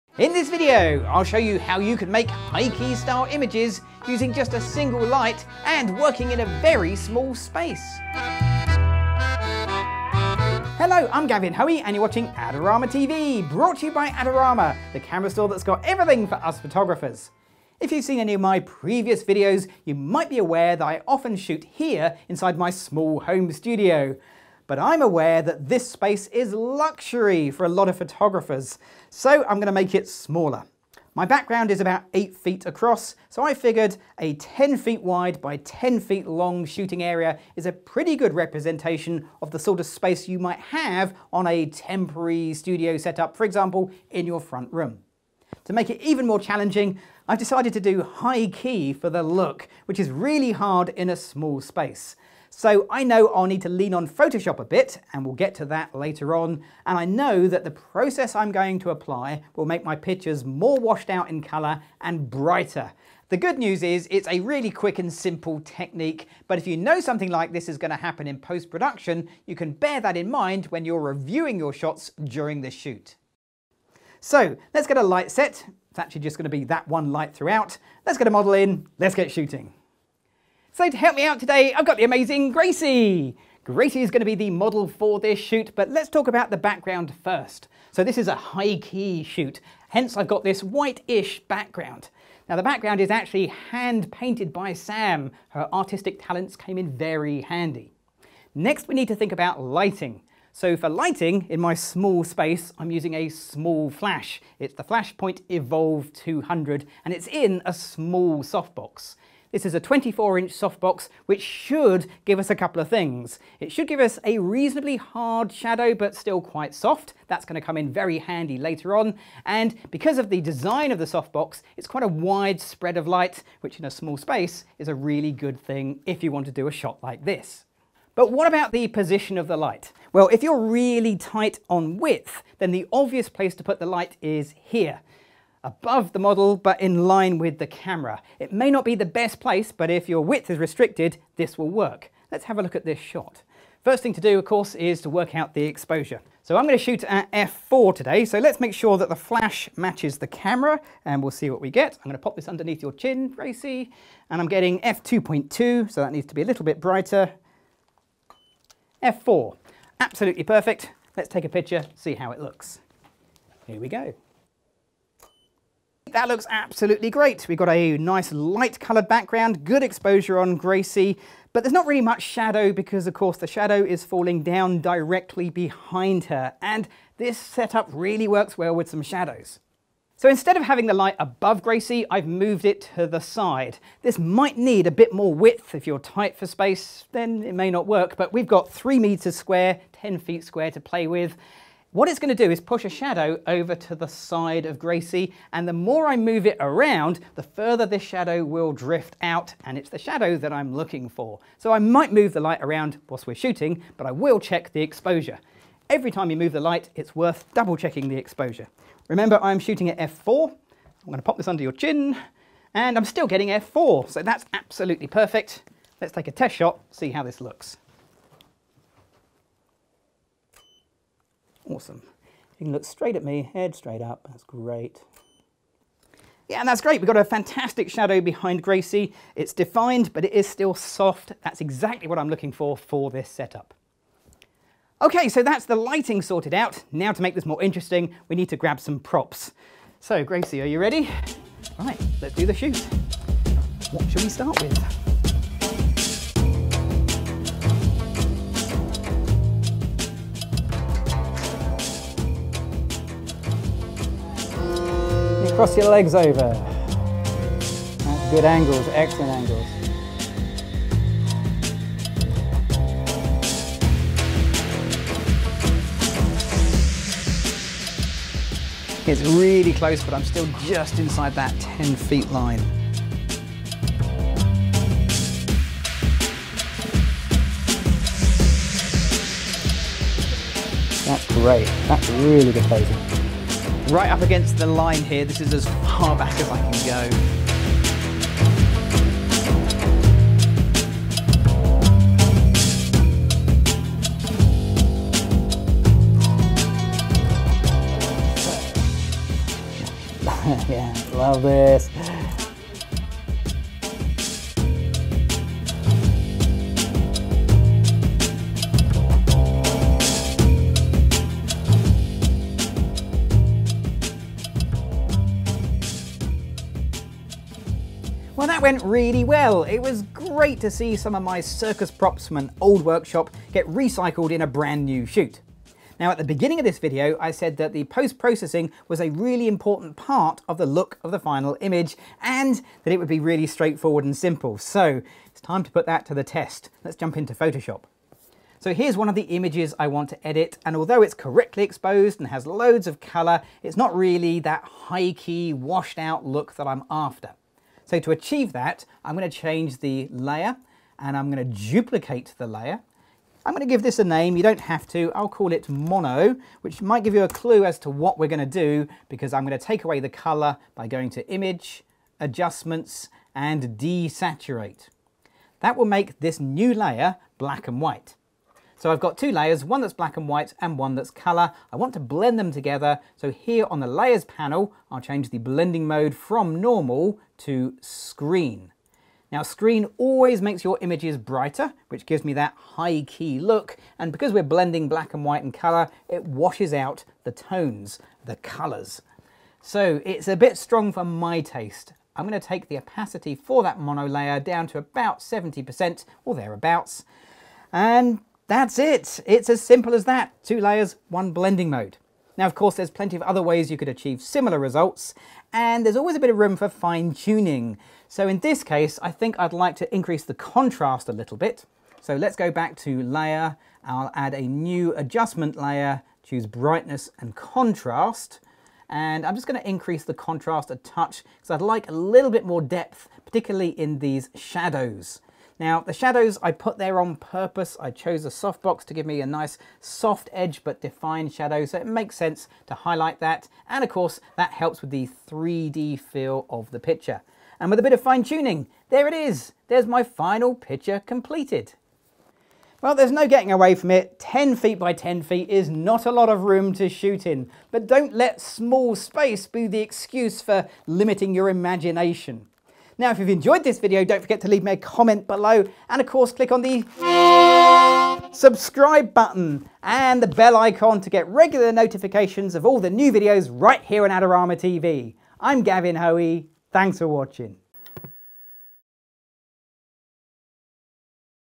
High Key In Small Spaces: Take and Make Great Photography with Gavin Hoey
Not many photographers are lucky enough to have a permanent studio set-up and ready to go. Using a room in your home as a temporary studio often means shooting space is at a premium and that can limit the sort of portraits you think you can shoot. So in this video Gavin Hoey sets out to make high key style images within a space that's only 10 feet by 10 feet (3 meters square).

The brushed effect background Gavin uses is homemade using grey studio background paper and basic white paint. During this tutorial Gavin shows the background being made and also how he adds the high key look with muted colors using a surprisingly simple Photoshop trick.